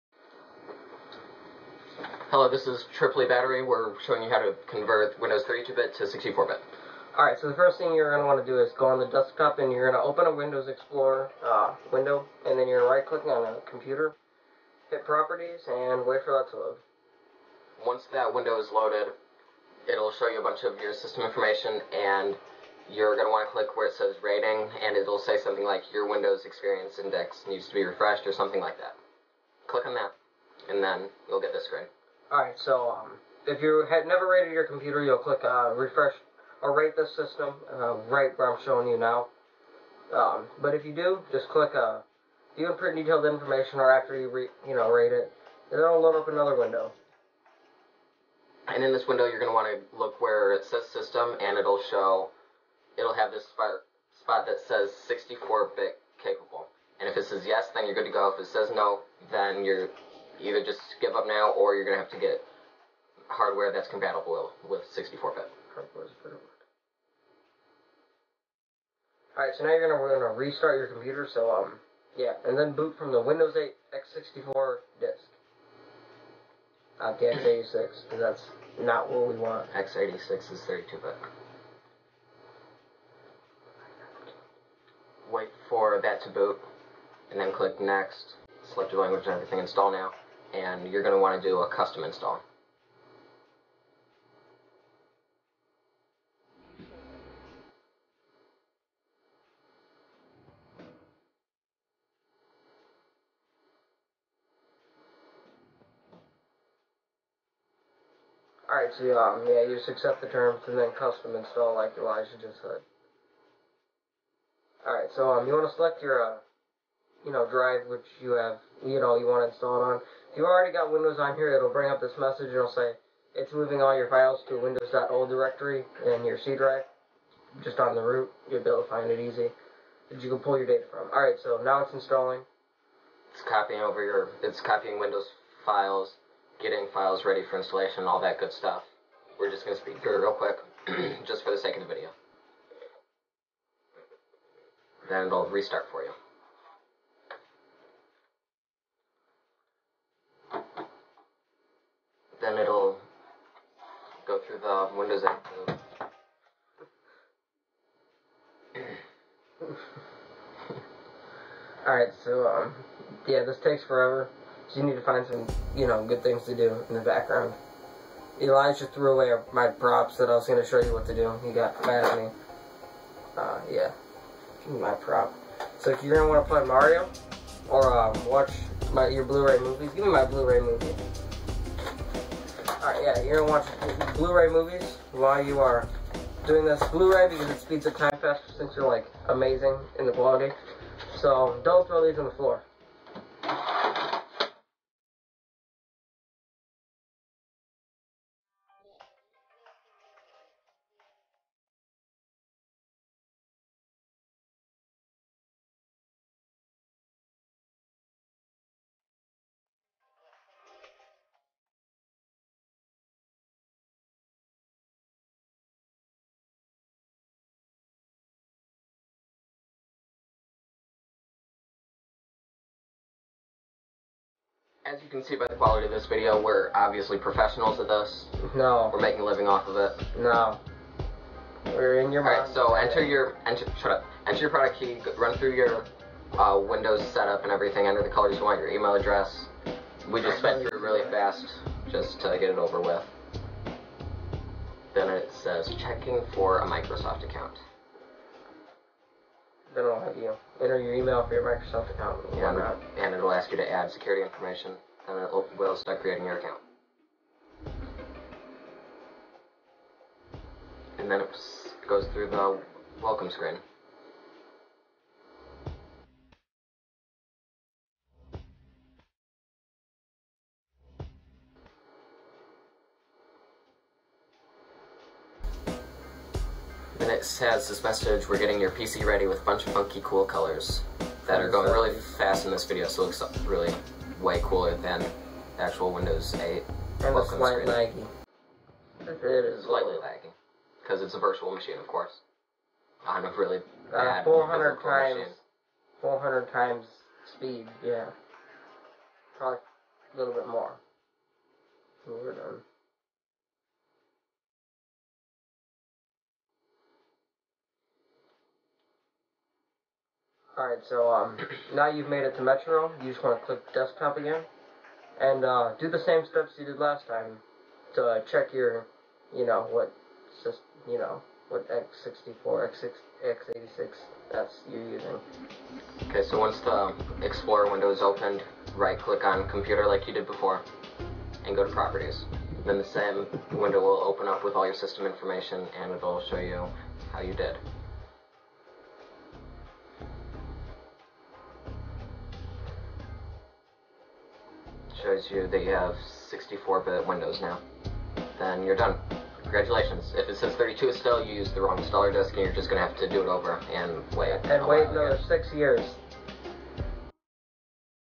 How To Upgrade Windows 32 Bit To 64 Bit Windows 7/8/10
Here's how to convert your 32 bit windows to 64 bit. For this demonstration we use Windows 8, but it will work on most versions of Windows without a problem.

All you have to do is make sure your computer is compatible with 64 bit by checking in your computer settings as shown in the video, and then install Windows 64 bit over your existing operating system.

Warning: This will clear any user settings, and programs. Your data will be backed up automatically to a Windows.old folder in the root of your C Drive.

Have fun taking advantage of your new 64 bit operating system!
Droid